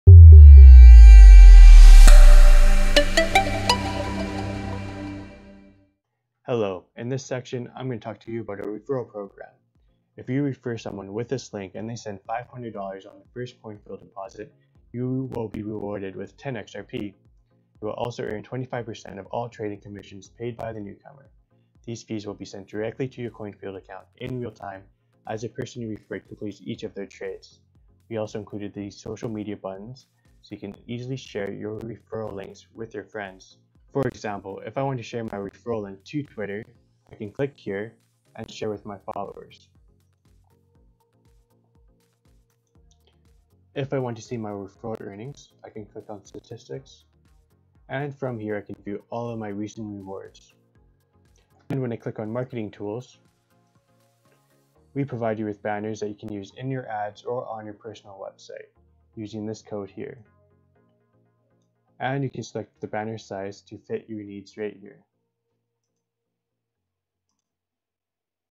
CoinField Review & Tutorial - Episode #9: Referral Program (Earn FREE XRP & 25% Commision)
Learn how to buy/sell Bitcoin, XRP, Ethereum and other cryptocurrencies on CoinField.

CoinField is the Easiest Way to Buy/Sell a Wide Variety of Cryptocurrencies in 101 Countries!